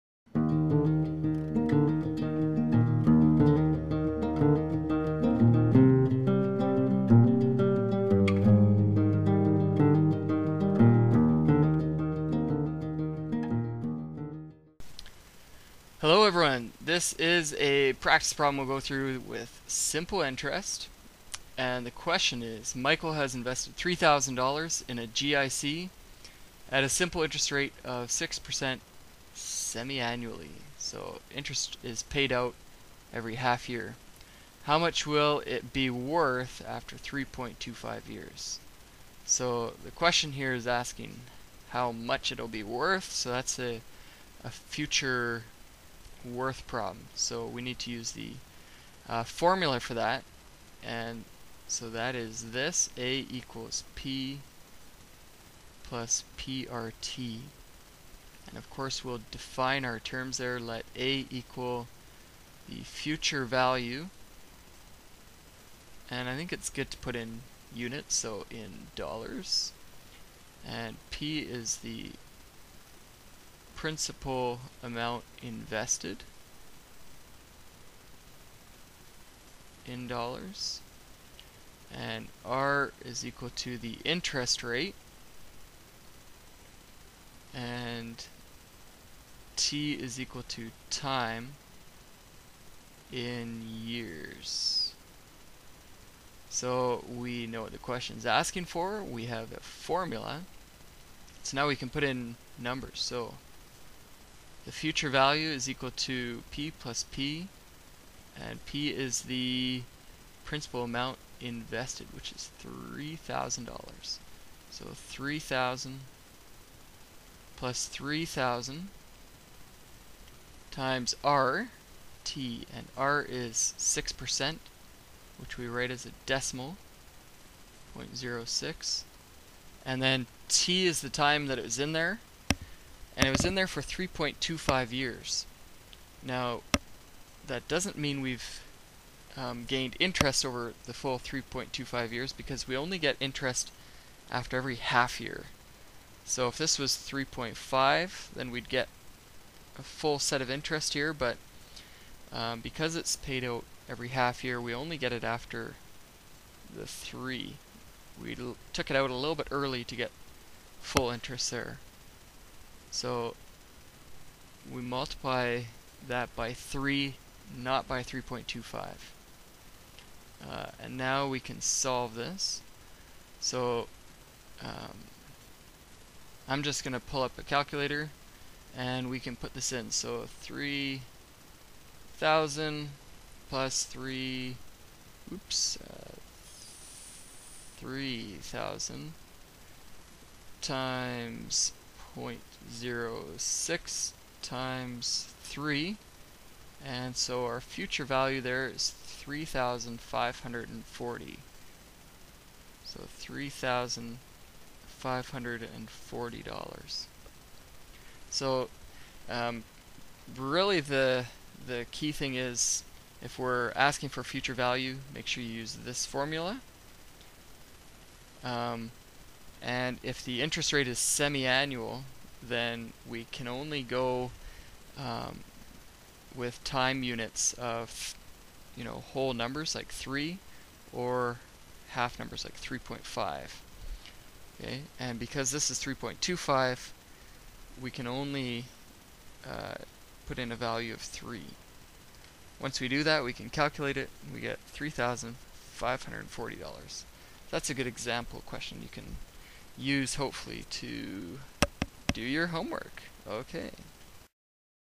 Lesson 1.1 (Foundations of Math 12)
Calculating simple interest for investments.